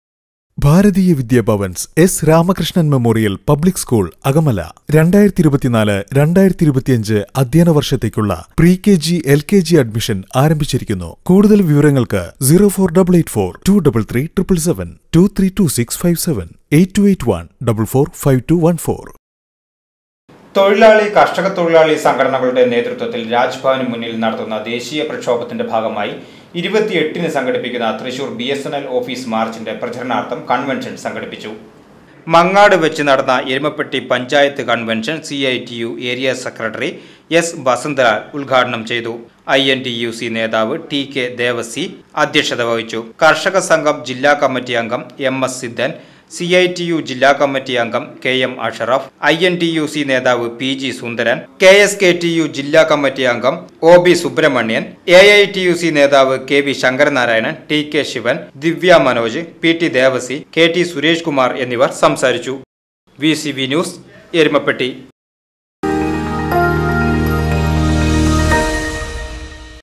തൃശ്ശൂർ BSNL ഓഫീസ് മാർച്ചിൻ്റെ പ്രചരണാർത്ഥം കൺവെൻഷൻ നടന്നു
ദേശീയ പ്രക്ഷോഭത്തിന്റെ ഭാഗമായി സംഘടിപ്പിക്കുന്ന തൃശ്ശൂർ BSNL ഓഫീസ് മാർച്ചിൻ്റെ പ്രചരണാർത്ഥം കൺവെൻഷൻ നടന്നു

VCV NEWS

VCV NEWS CHANNEL
Wadakkanchery, Thrissur